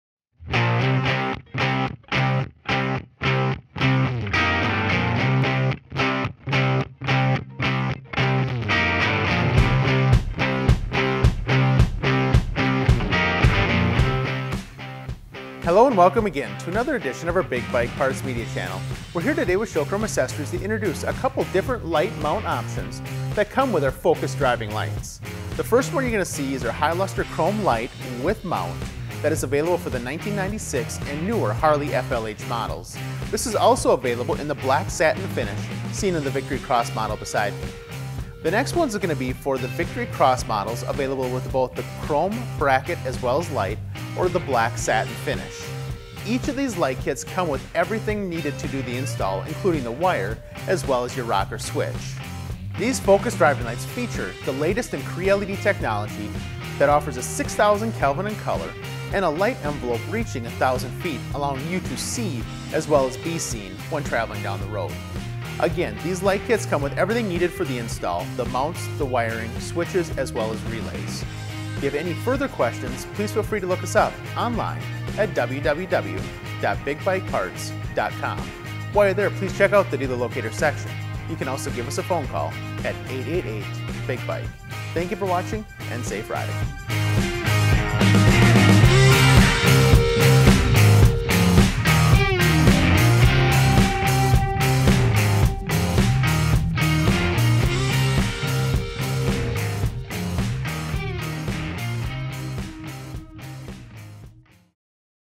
LED Driving Light Kits in Chrome or Black Satin for Harley-Davidson and Victory
Setting the standard with their all new 3½’’ LED driving lights, Big Bike Parts® gives you a choice of chrome or black satin for Harley and Victory riders.  Custom fairing mounts for bike specific fitments, you can choose Big Bike Parts® new  3½’’ visored Hi-Intensity driving lights with dual CREE LEDs per light in either an all chrome finish or black satin.  With 6000K brightness, these lights are not only bright, but have an exceptional 1000 foot light envelope.  Fitments for the Harley FLH and Victory Cross series of bikes.

Part # 91-315L   3½’’ Driving Kit FLH 1996-Newer
Part # 91-315LBK   3½’’ Black Driving Kit FLH 1996-Newer

Part # 30-110L   3½’’ Driving Kit Victory Cross, etc
Part # 30-110LBK    3½’’ Black Driving Kit Victory Cross, etc